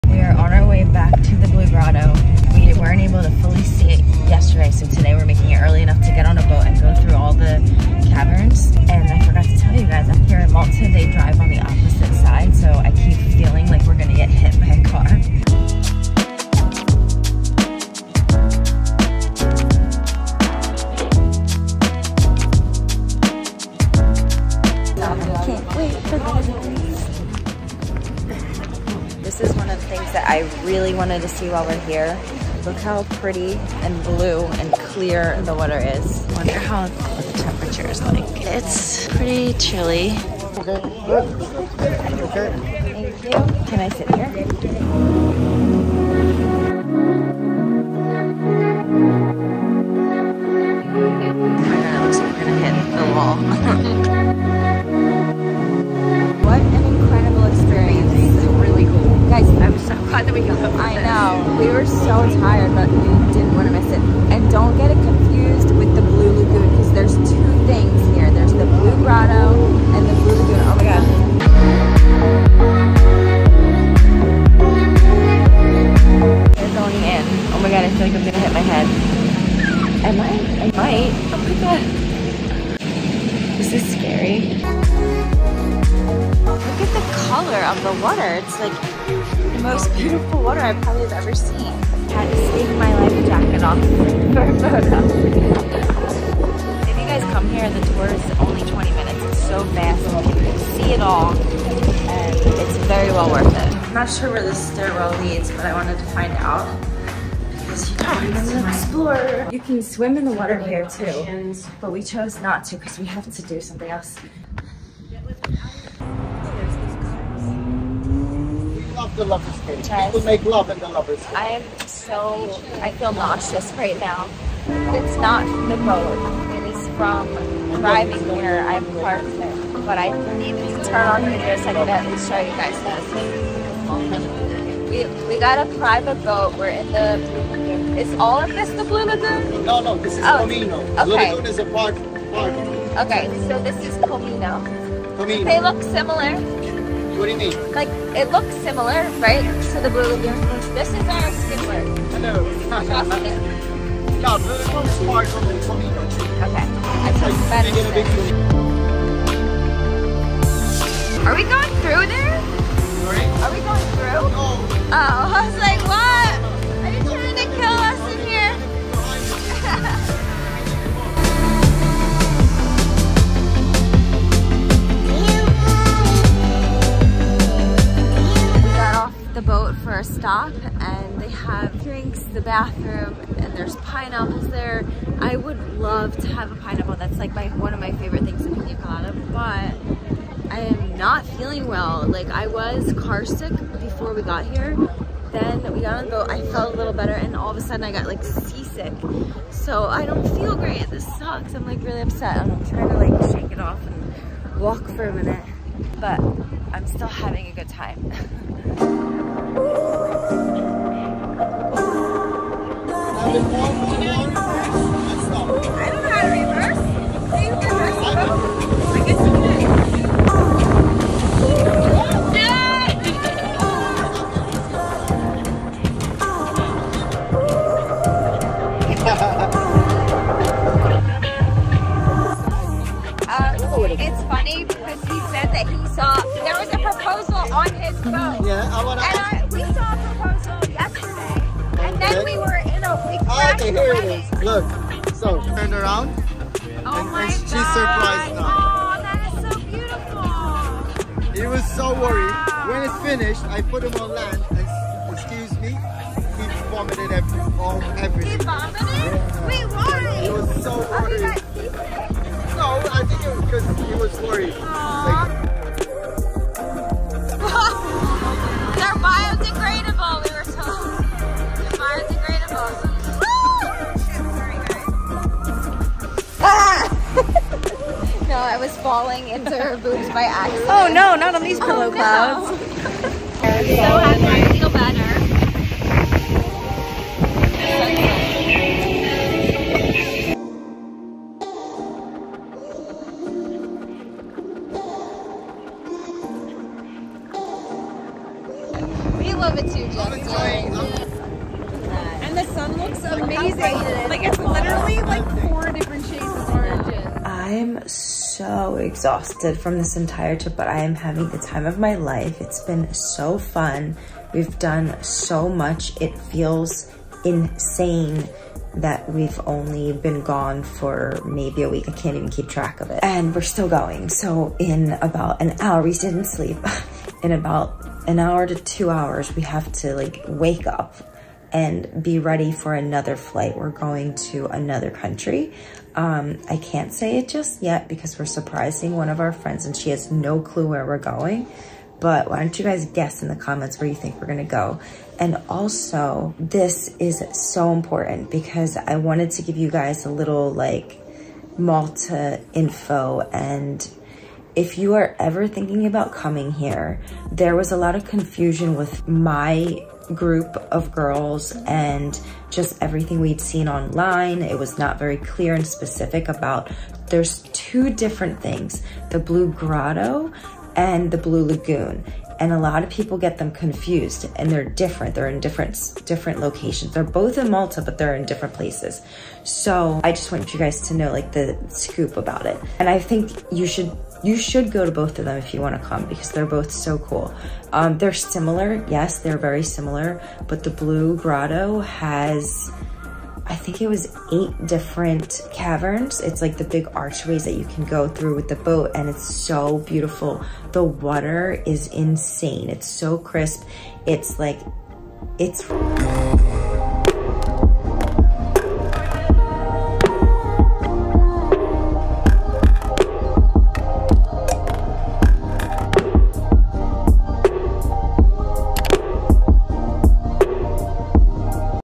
BLUE GROTTO vs BLUE LAGOON
A lot of people get the Blue Grotto and Blue Lagoon confused. They are two separate locations in Malta and they do have their beautiful differences. Watch till the end to see what makes them each unique!